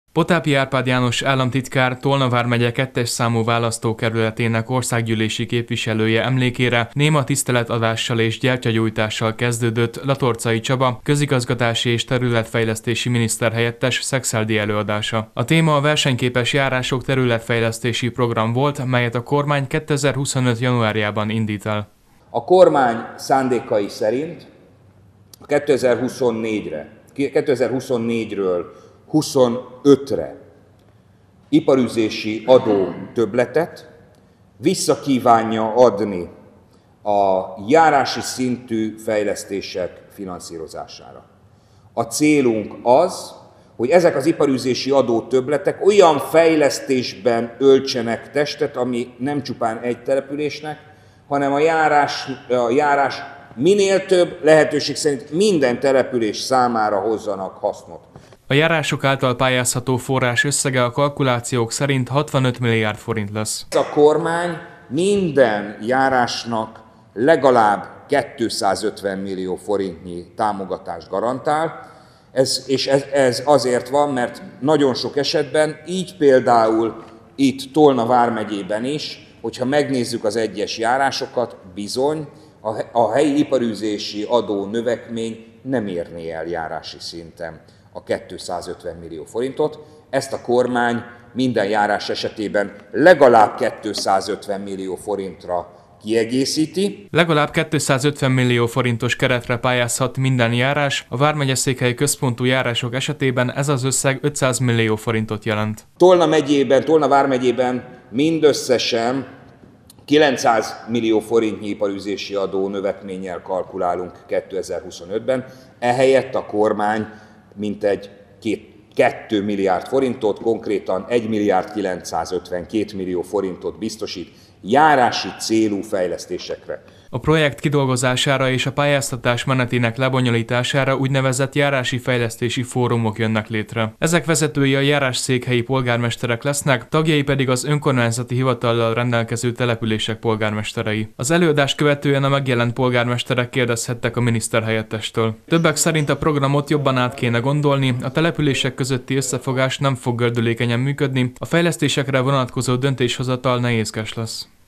Versenyképes Járások Program - Latorcai Csaba ismertette a projektet Szekszárdon
Magyarország Kormányának döntése értelmében 2025 januárjától új területfejlesztési program indul Versenyképes Járások Területfejlesztési Program néven, a központi és a vidéki térségek fejlettségi különbségeinek mérséklése, a vidék népességmegtartó erejének növelése érdekében. A programmal kapcsolatban Latorcai Csaba, közigazgatási és területfejlesztési miniszterhelyettes tartott előadást Szekszárdon a Vármegyeházán.